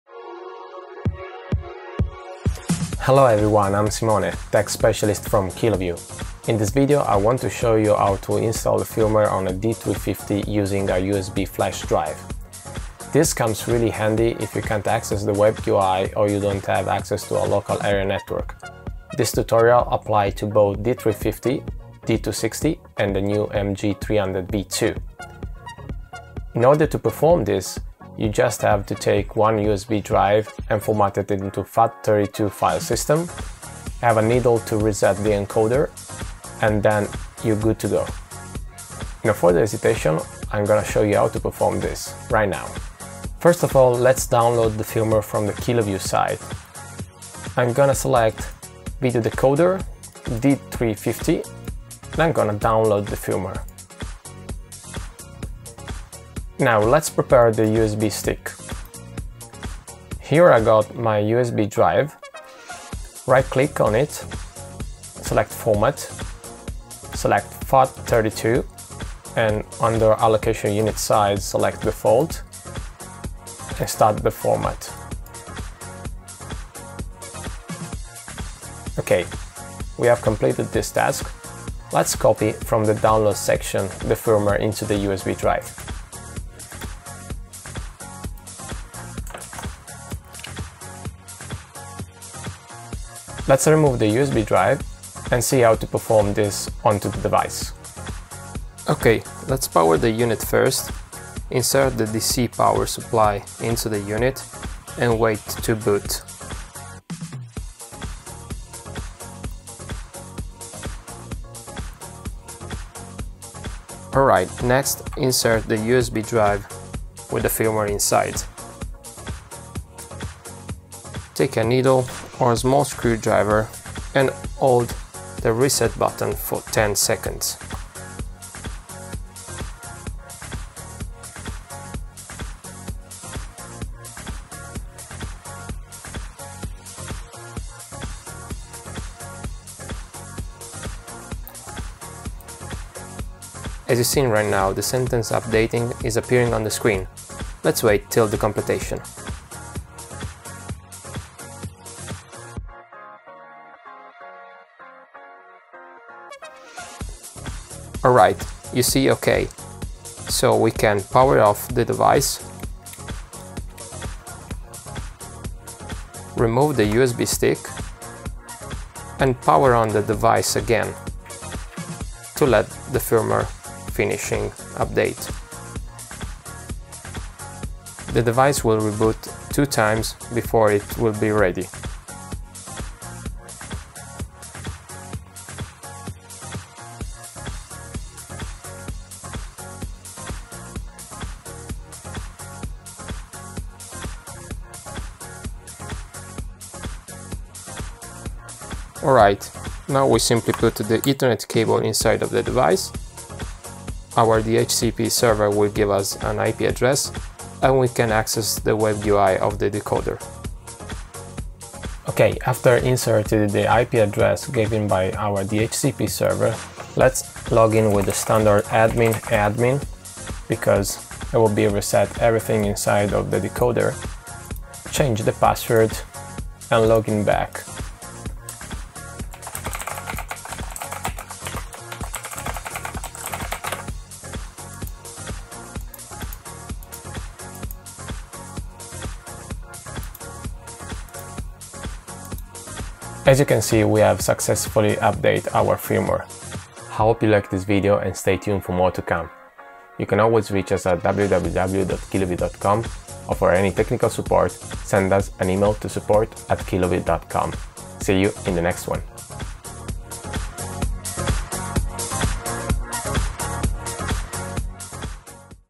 USB FIRMWARE FLASH RECOVERY DECODERS & GATEWAY
Ready to level up your tech game? Join Kiloview's Simone as he breaks down the essentials of firmware downgrading and recovery. From legacy devices to the latest NDI series, he will got you covered with practical insights and tips. Don’t let outdated tech hold you back—watch now and unleash your device’s full potential!